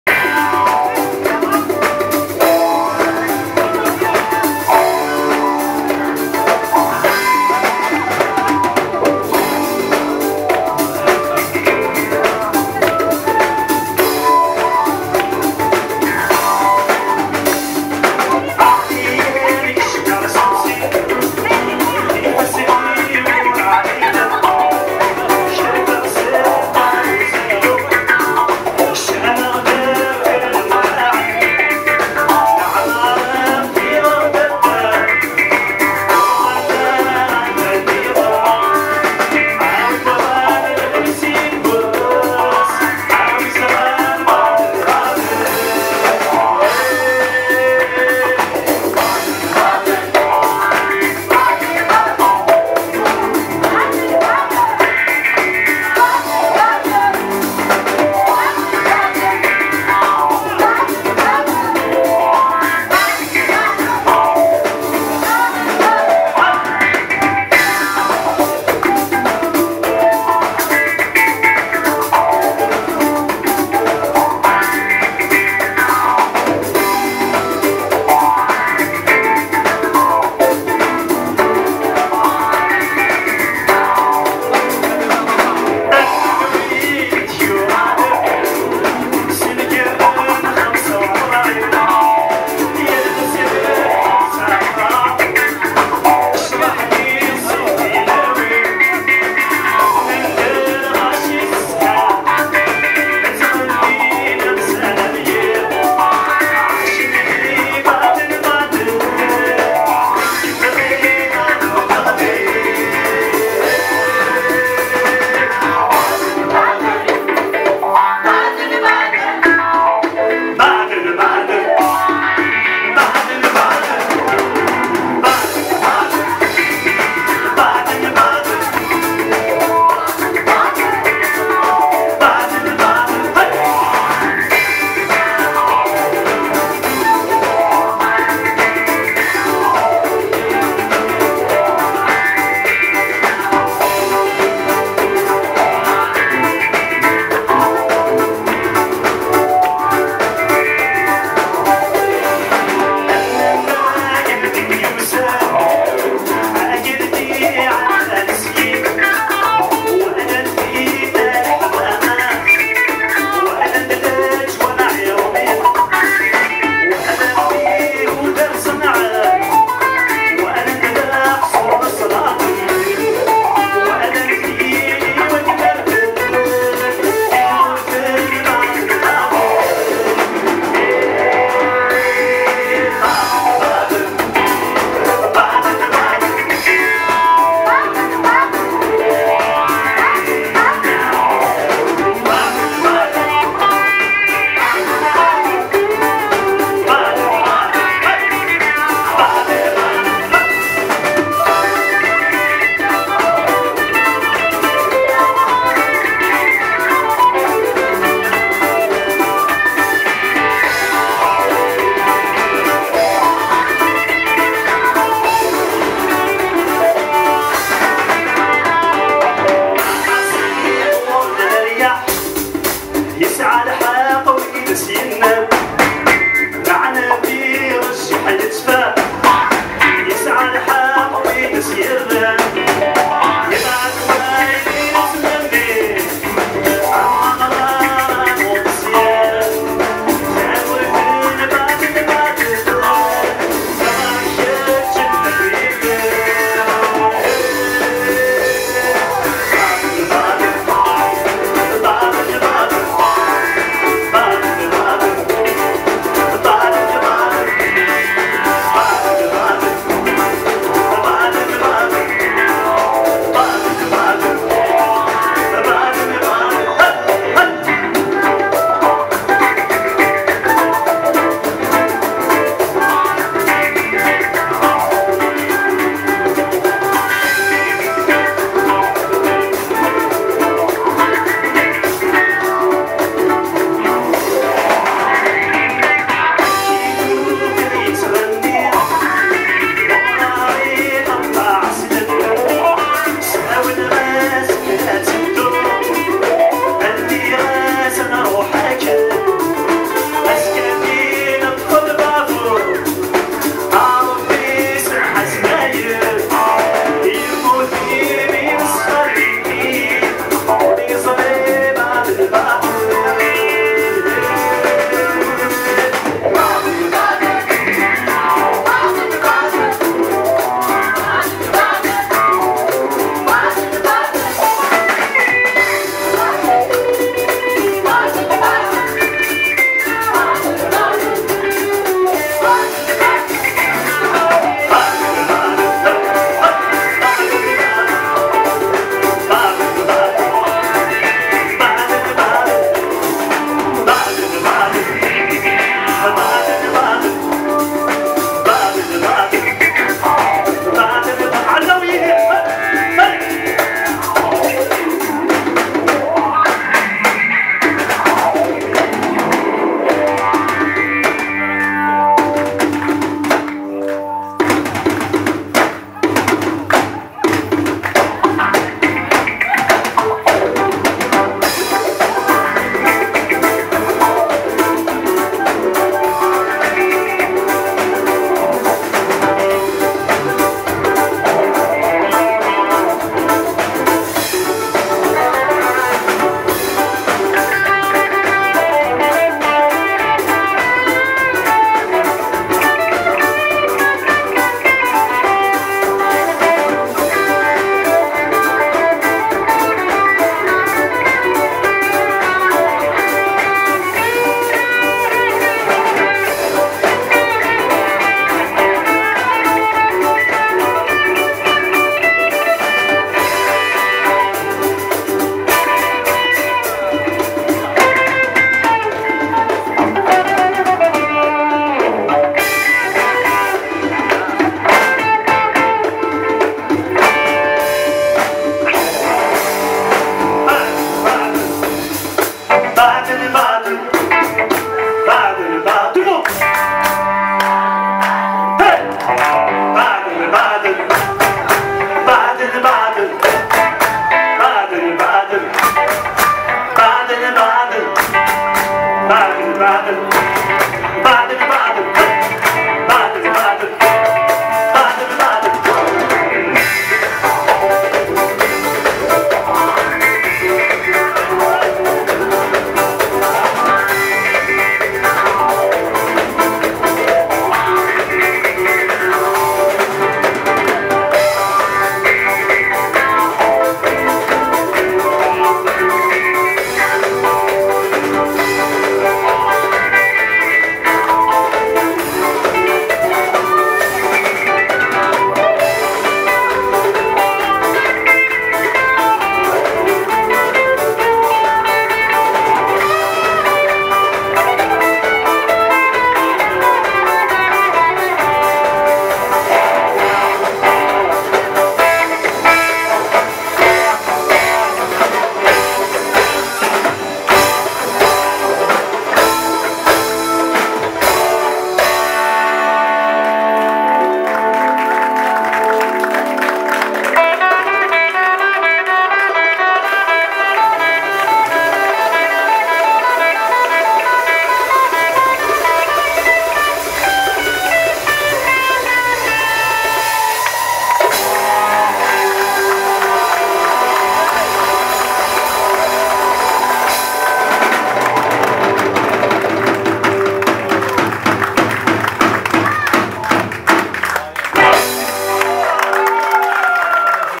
Baden-Baden live de Zayen(on danse on chante et on s'amuse)
Zayen chanteur auteur-compositeur-Kabyle